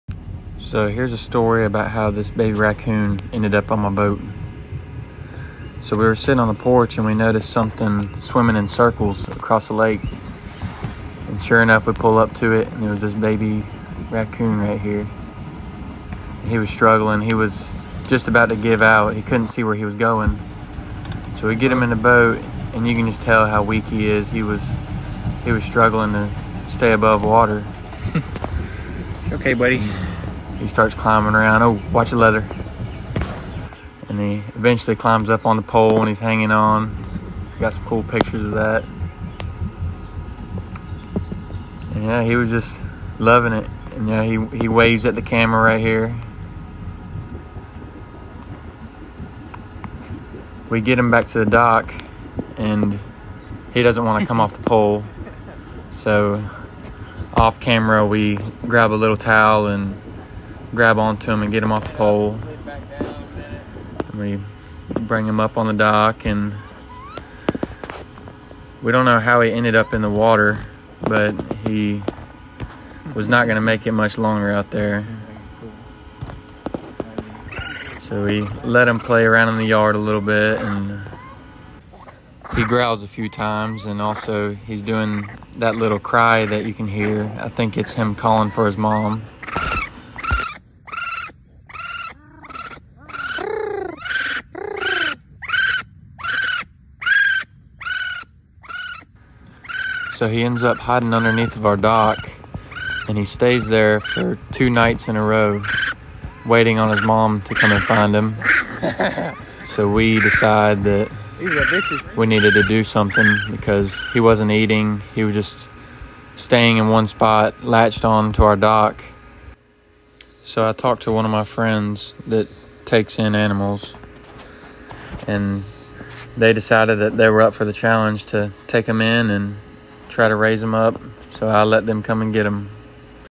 Kindhearted man saves scared baby raccoon from drowning in lake
Ryan is sitting on his porch in Charlotte, North Carolina when he sees something "swimming in circles across the lake". He takes his boat out to investigate further, and finds a baby raccoon struggling in the water. Ryan saves the baby raccoon and brings it into his boat.

He steers the boat back onto the dock but the baby raccoon is reluctant to get off the boat. He ends up having to grab the raccoon off the boat himself and lets it down on some grass. The scared baby raccoon ends up hiding underneath the dock for two days without food or water.

Ryan is determined to get the distressed raccoon some help, so he has one of his friends who is experienced in rescuing animals.